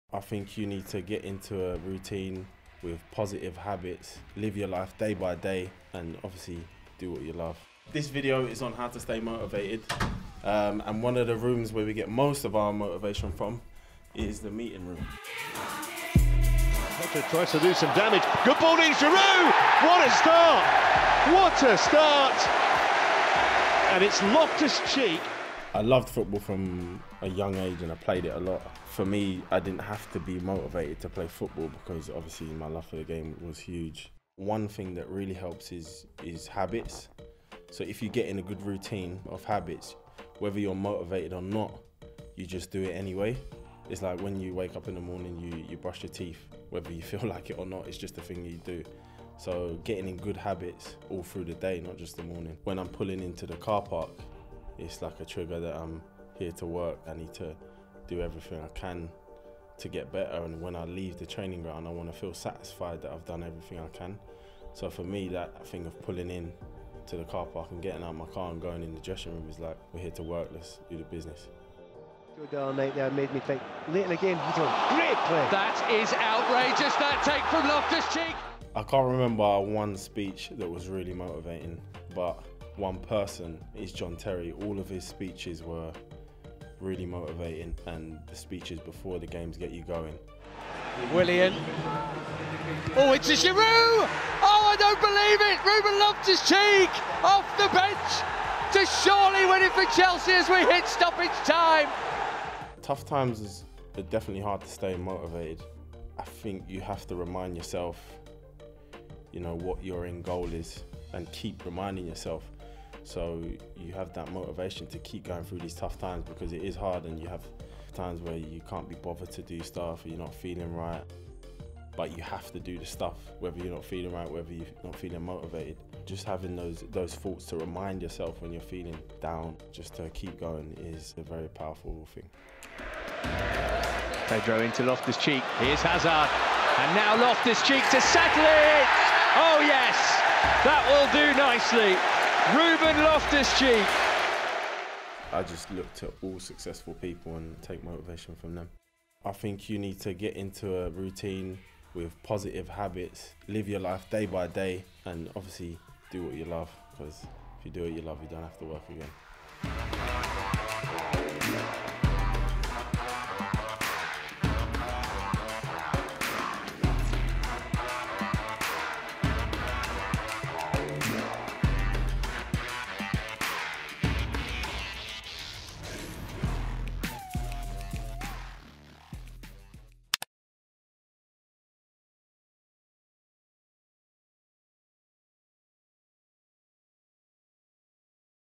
Ruben Loftus-Cheek On His Road To Recovery | Train Like A Pro
Ruben Loftus-Cheek tells all about how he stayed motivated through his recovery period and reveals the key steps to getting match fit.